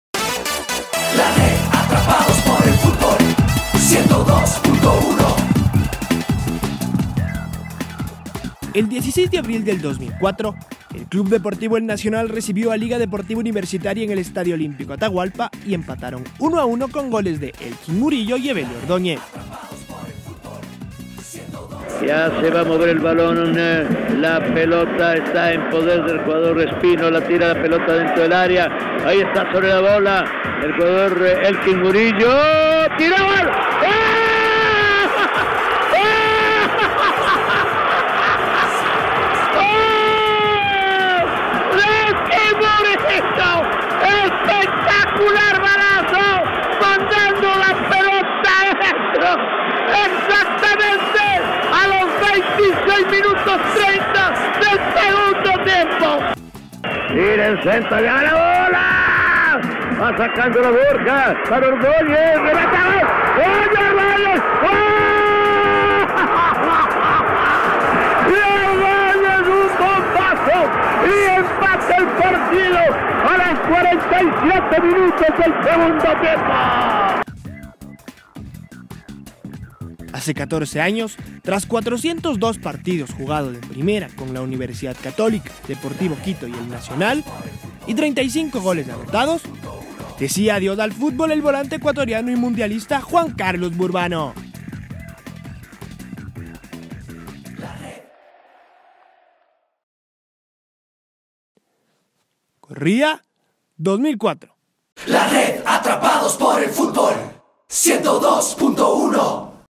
REDiario | ABR | ¡Juan Carlos Burbano jugaba su último partido en nuestro fútbol!
REDiario | ABR | ¡Juan Carlos Burbano jugaba su último partido en nuestro fútbol! ¡'Criollos' y 'albos' empataban 1-1 en el Atahualpa!